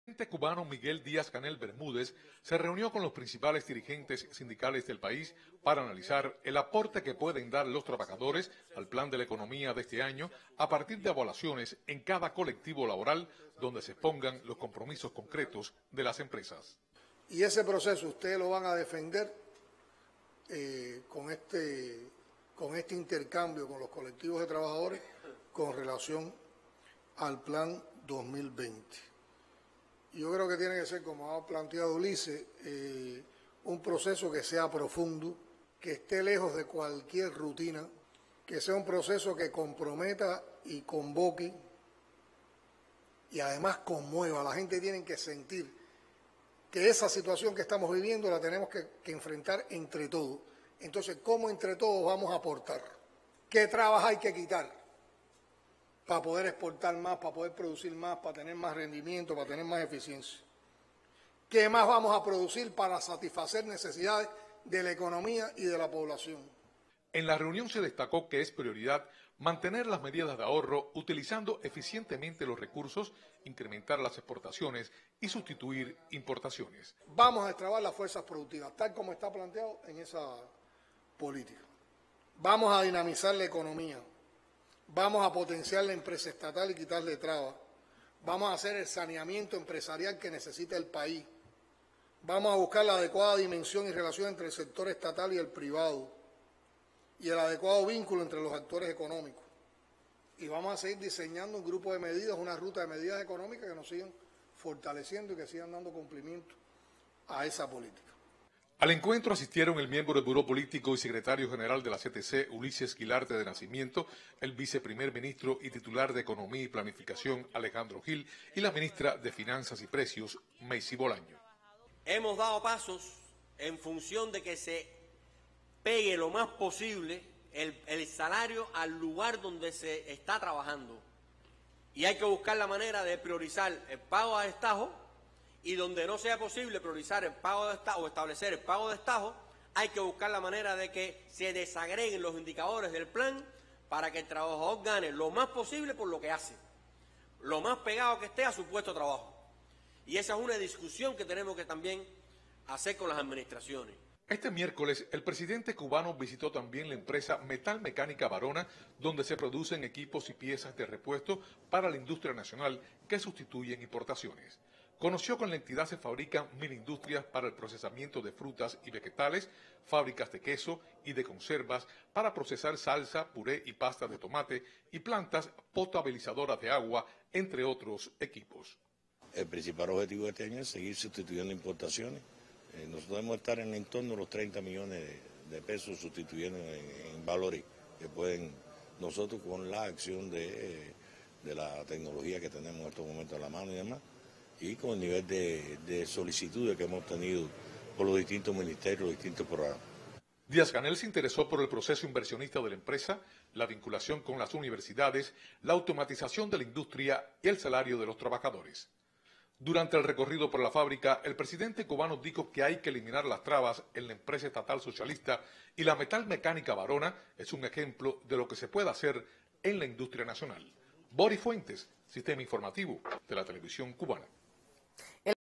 Sostiene presidente Díaz-Canel encuentro con líderes sindicales de Cuba
El presidente de la República de Cuba: Miguel Díaz Canel Bermúdez sostuvo un encuentro importantes líderes sindicales cubanos.

En el encuentro sostuvo intercambio sobre asuntos medulares de la industria cubana. Además, participaron los titulares cubanos de Economía y Planificación y Finanzas y Precios.